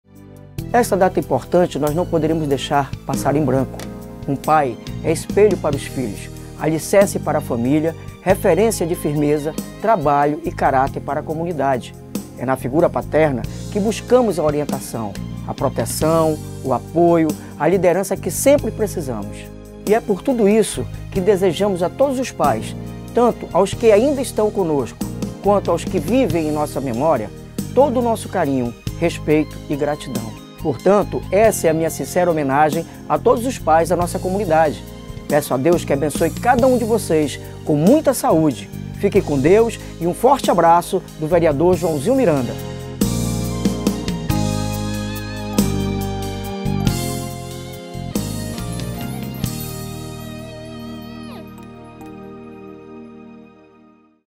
Homenagem ao dia dos Pais
O vereador Joãozinho Miranda, faz uma parabeniza os pais no seu dia.